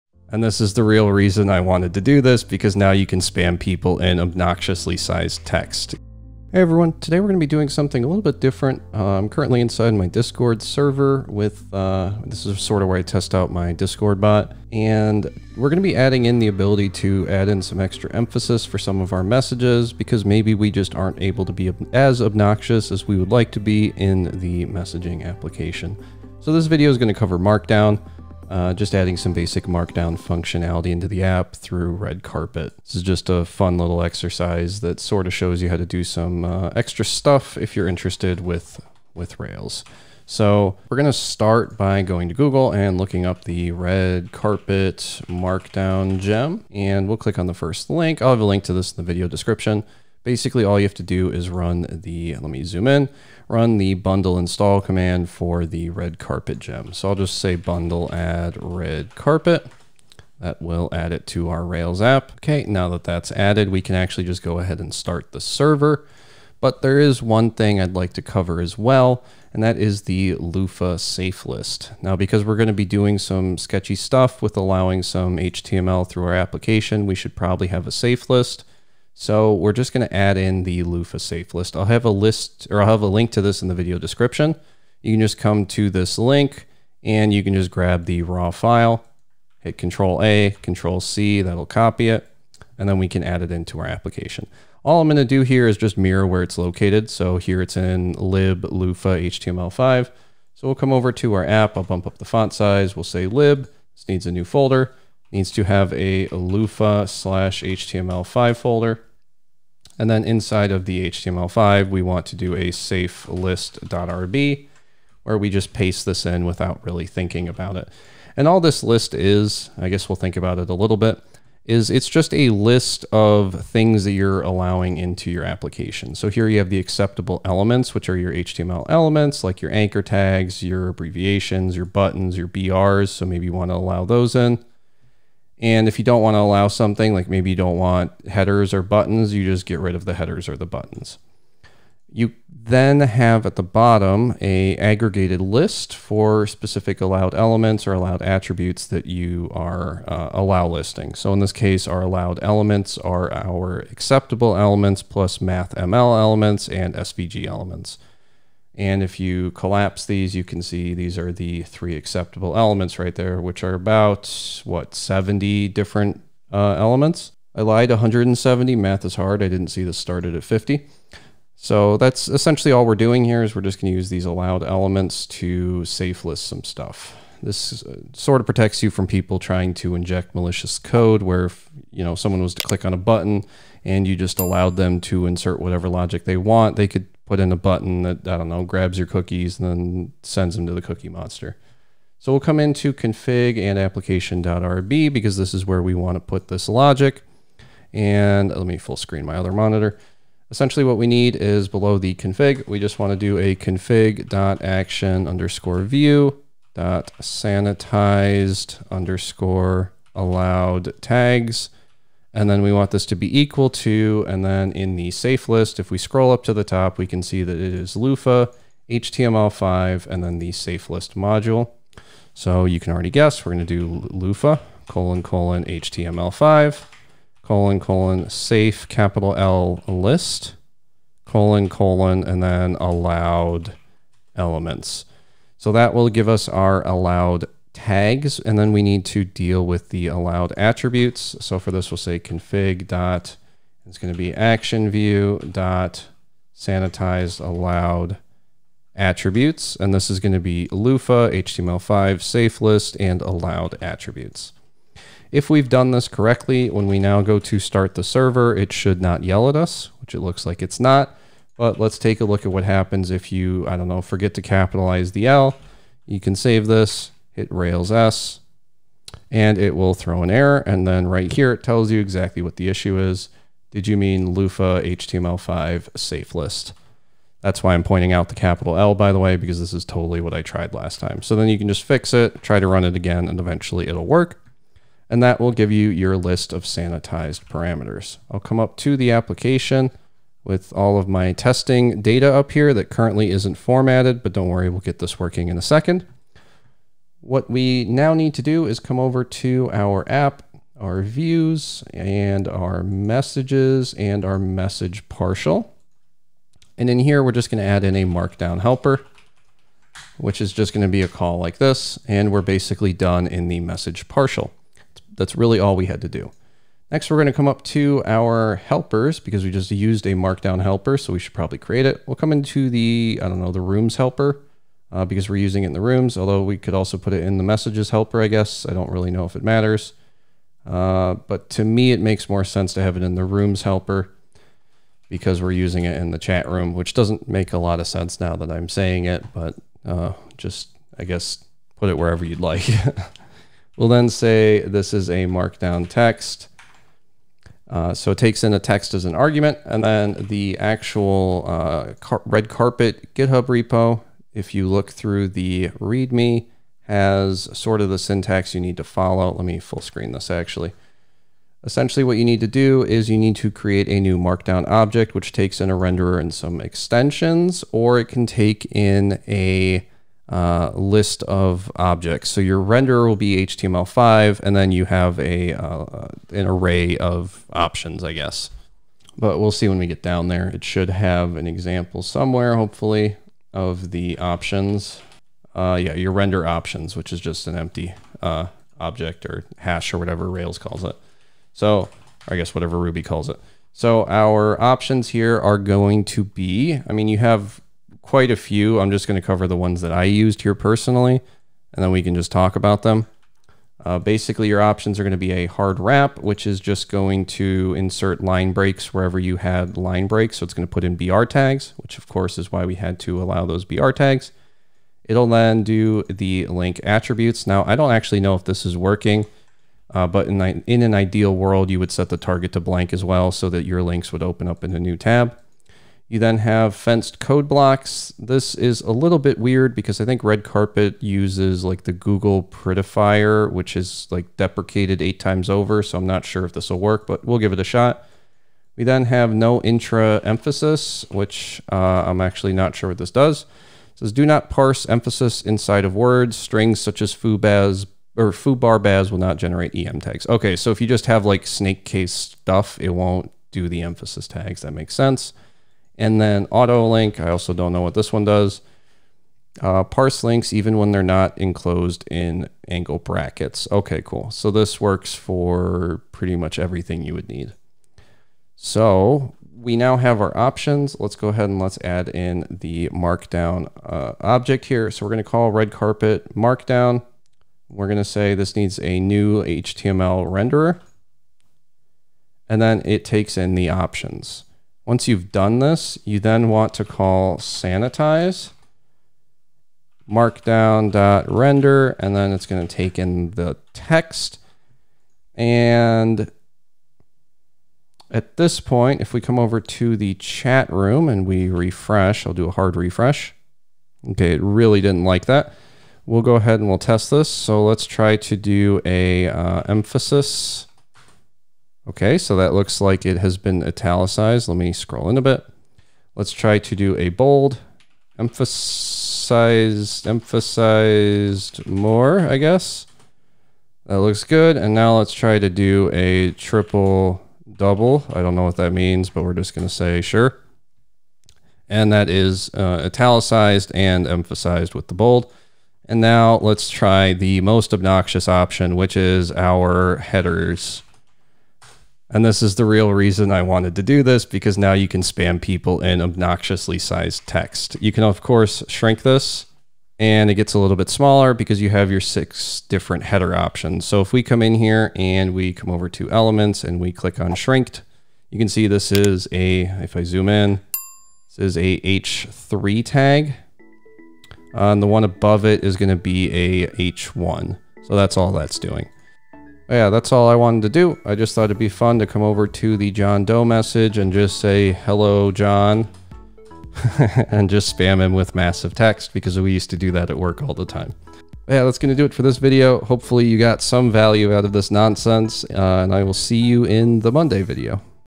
Discord Markdown In Rails 7 (Probably Clickbait) | Turbochat Part 18
Let's implement Discord Markdown so that we can spam our friends! :)

Timestamps
0:00 Looking At Discord's Markdown
0:39 Adding Redcarpet Gem For Markdown
1:02 Adding Loofah Sanitizer Tag List
5:00 Redcarpet Markdown Implementation Rails 7